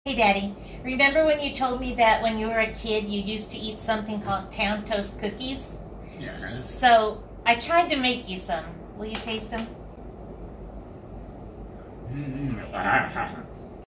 GISH 2019 Item 102 Senior Memory Snacks
Greatest International Scavenger Hunt 2019
Reach out to a senior in your family or community and ask them about a commercially made snack or dish from their childhood that brings them fond memories—something that just doesn’t exist anymore. Try to recreate it based on their description, then share your creation with them and record their assessment.